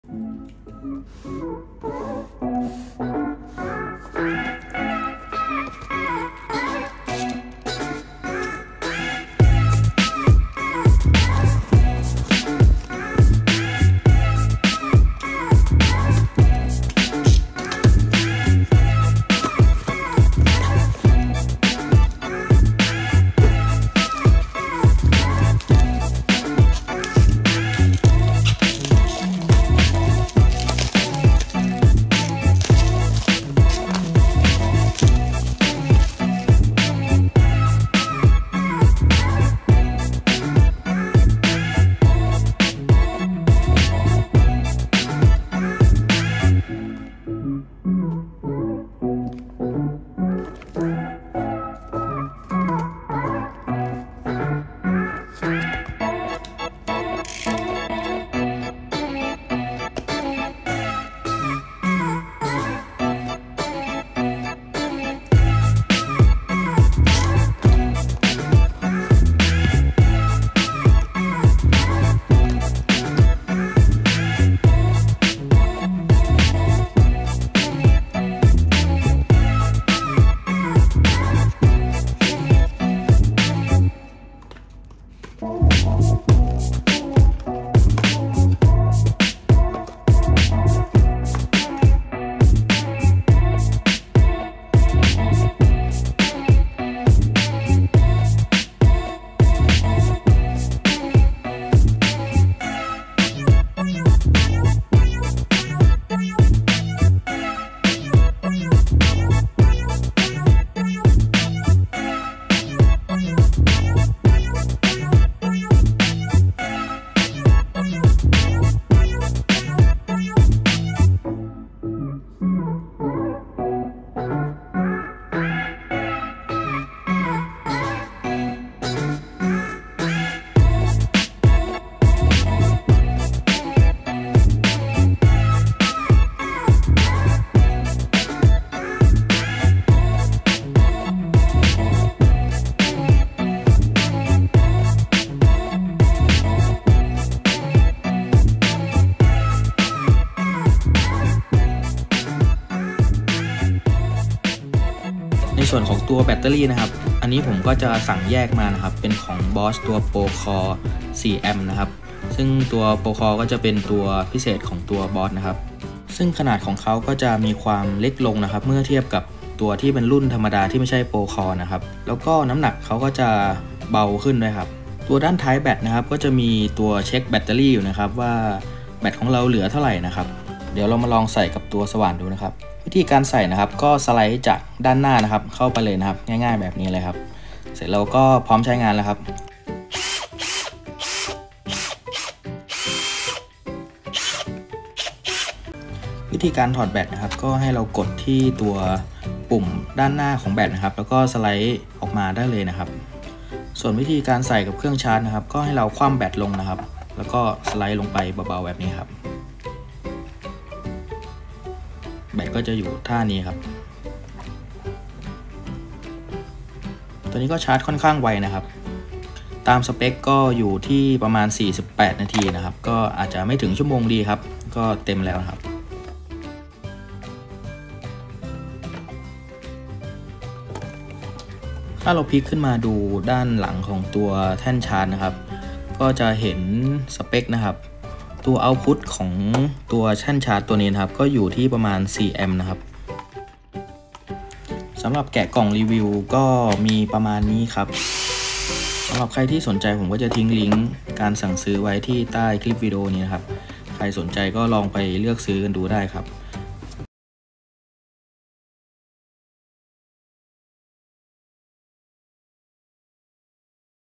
BOSCH BATTERY PROCORE 18V 4.0 AH แบตเตอรี่ ความจุสูง ของแท้
เพิ่มประสิทธิภาพแบตเตอรี่ ในน้ำหนักและขนาดกะทัดรัด

 - เทคโนโลยีเซลล์ล่าสุดให้พลังงานเท่ากับแบตเตอรี่ 4 Ah มาตรฐาน แต่มีน้ำหนักน้อยกว่าและมีขนาดเล็กกว่าอย่างเห็นได้ชัด
 - Flexible Power System: ใช้ร่วมกับเครื่องชาร์จและเครื่องมือไฟฟ้า 18 โวลท์ของ Bosch Professional ทั้งรุ่นที่มีอยู่และรุ่นใหม่ได้  100%
 - อายุการใช้งานยาวนานขึ้น 135% เมื่อเปรียบเทียบกับแบตเตอรี่มาตรฐาน เนื่องจากมีเทคโนโลยี COOLPACK 2.0 ที่ช่วยป้องกัน แบตเตอรี่ไม่ให้มีความร้อนสูงเกิน